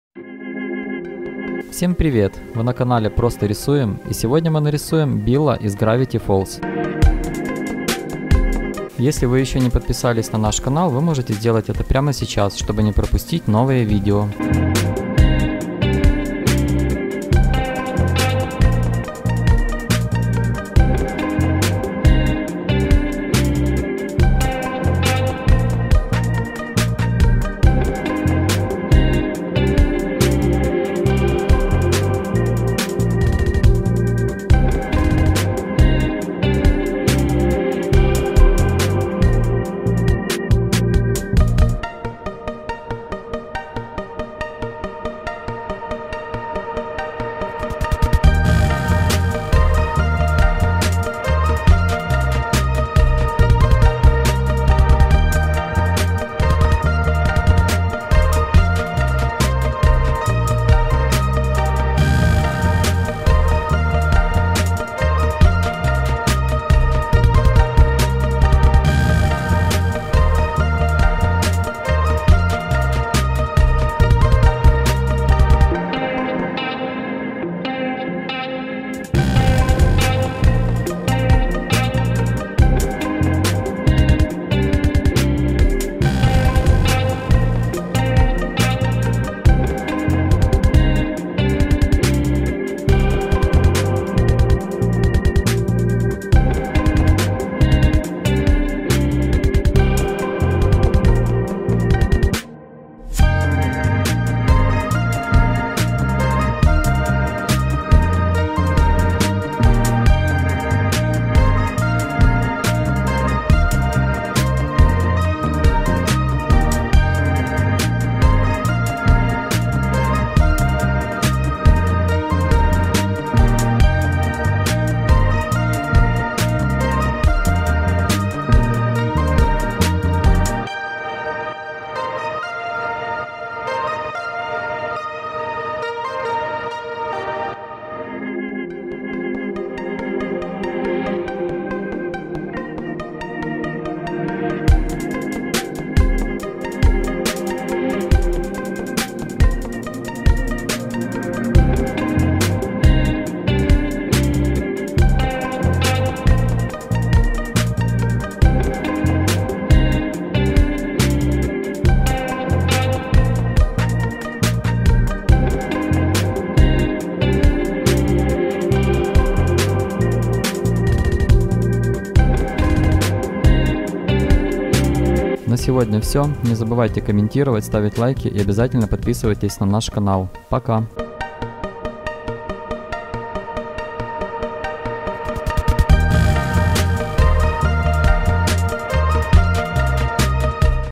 Как нарисовать, БИЛЛ ШИФР из Гравити Фолз, рисунки для детей и начинающих
Привет) В этом видео вы увидите как нарисовать и раскрасить мультперсонаж БИЛЛ ФОЛЗ просто, поэтапно, шаг за шагом. Рисунки Гравити Фолз.